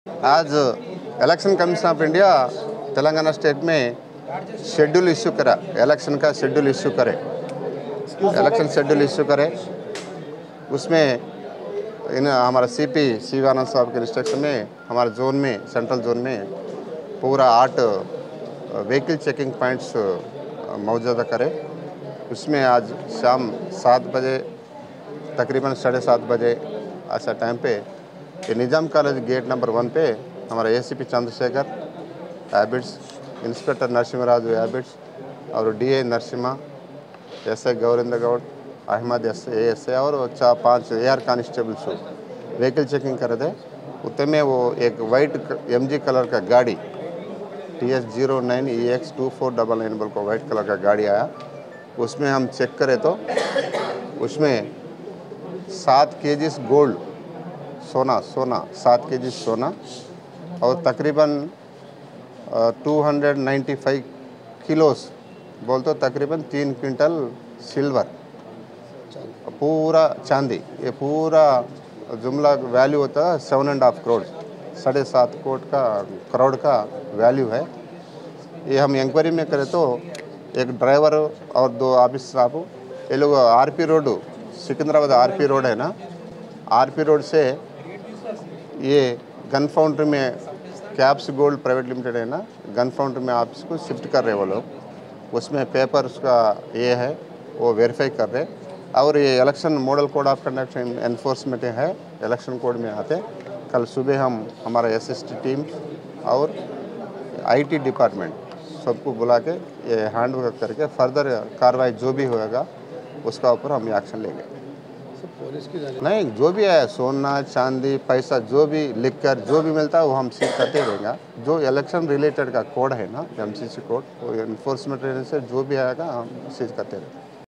Nizam College Ki Gate Ke Pass Checking...
Nizam College Ki Gate Ke Pass Checking Ke Dauran Police Ko 7_Kilo_Gold Aur 300_Kilo_Chandi Ke Zeevrat Huee Buramad...